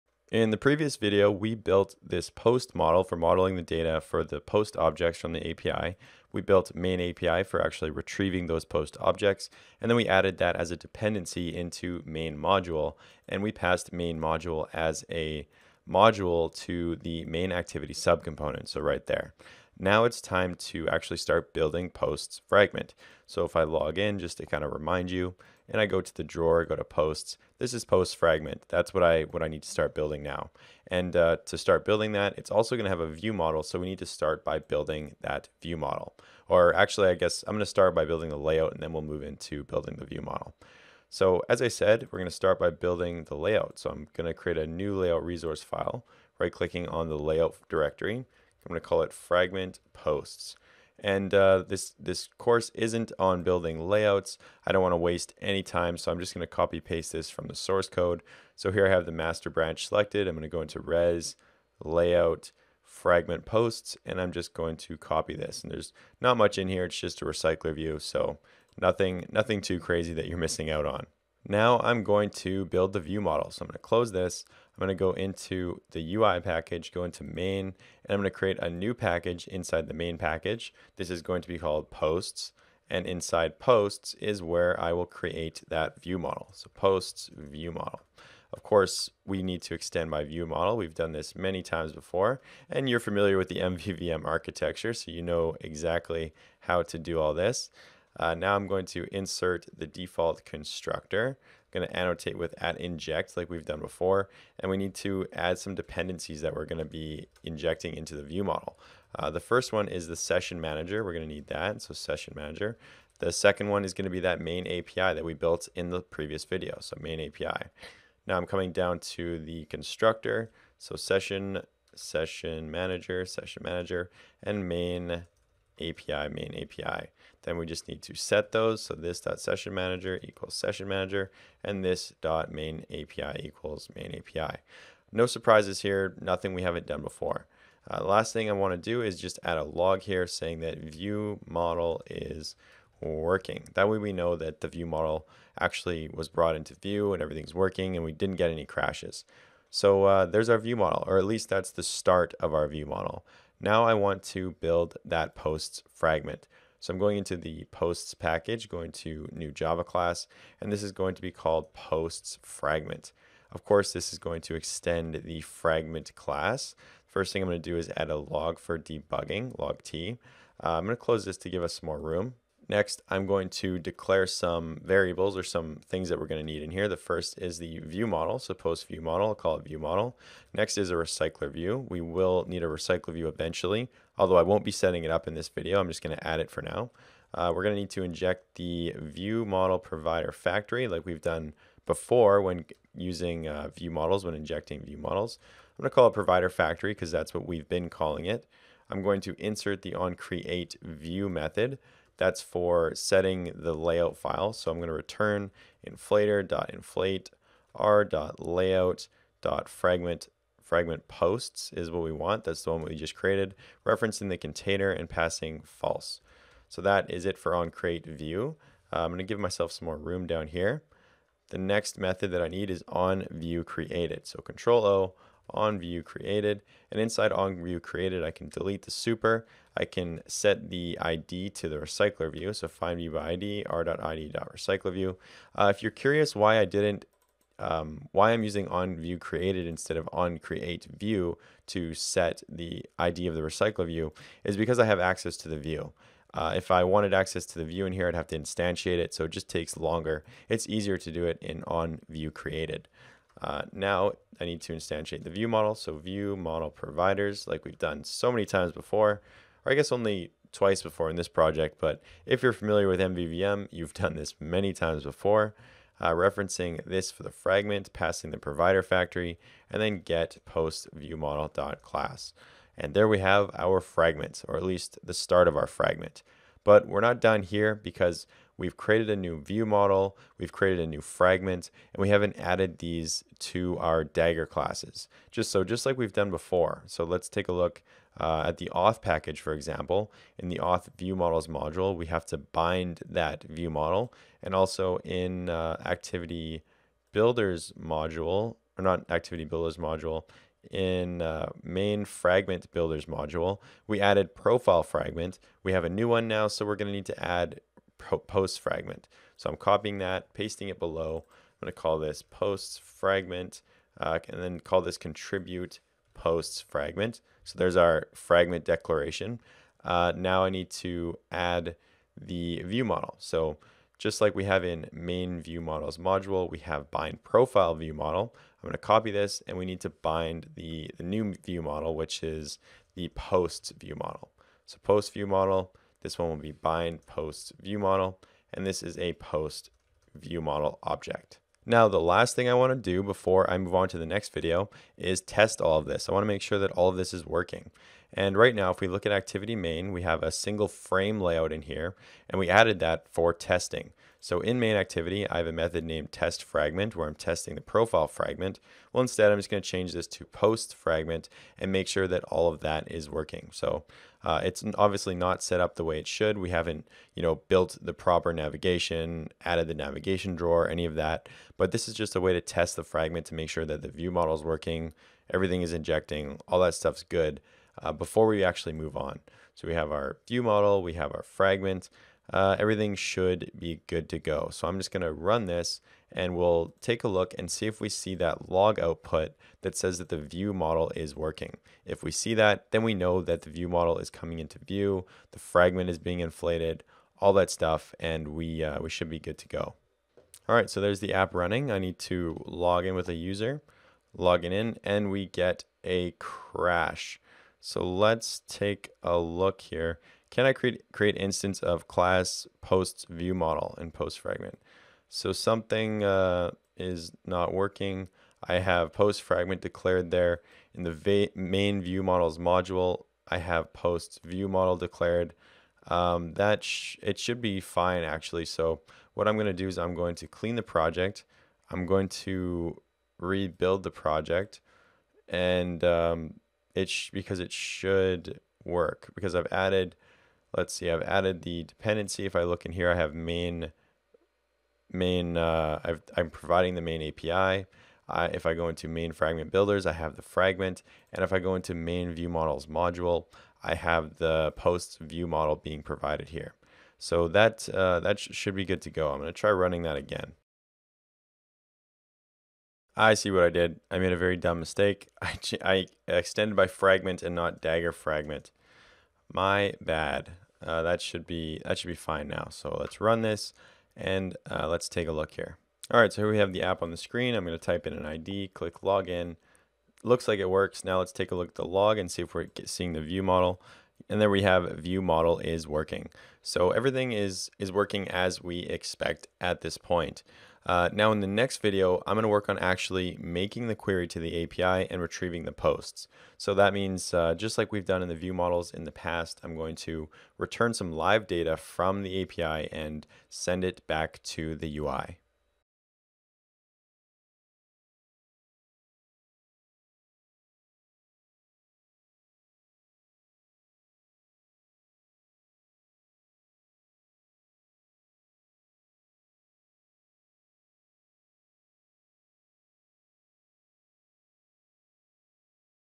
Subcomponents, ViewModels, and Fragments with Dagger 2
In this video I'm going to start building PostsFragment, and the ViewModel that goes along with it.

The cool thing about PostsFragment is that it will use dagger dependencies that are only accessible inside the MainComponent, which is a subcomponent.